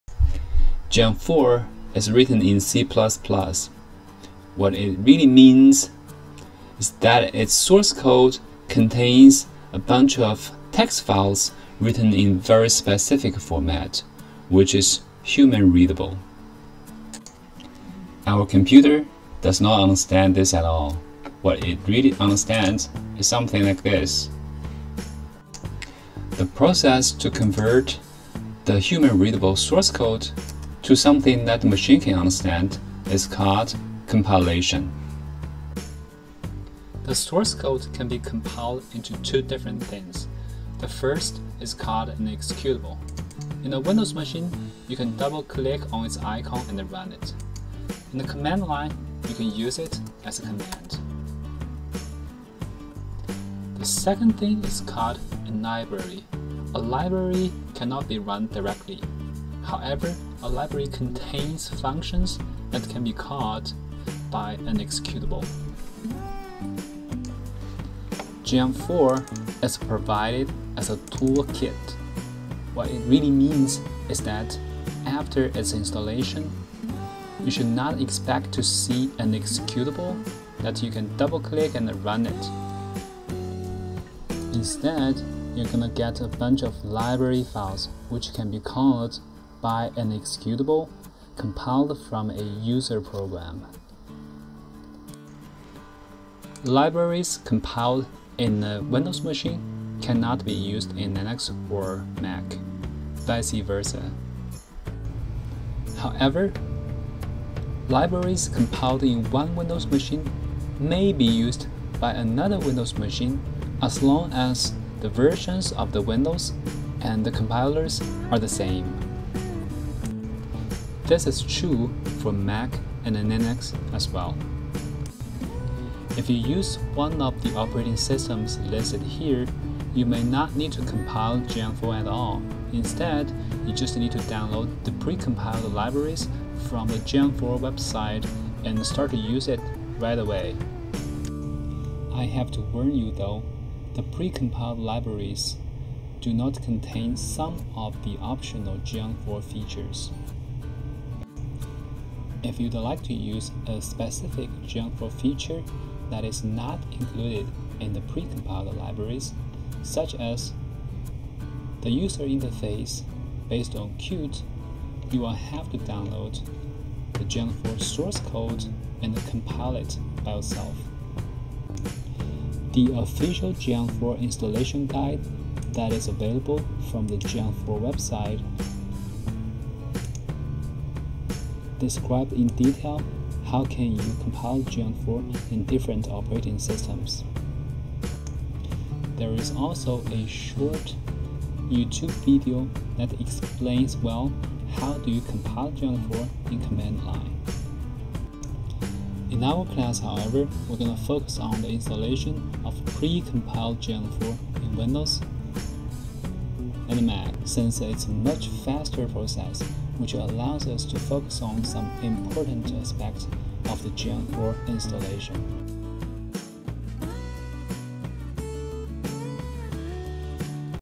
1. Compile or not, it's a question (Hands-on Geant4 Mini Course)
Let's understand whether it is necessary for you to compile Geant4 libraries in some operating systems. References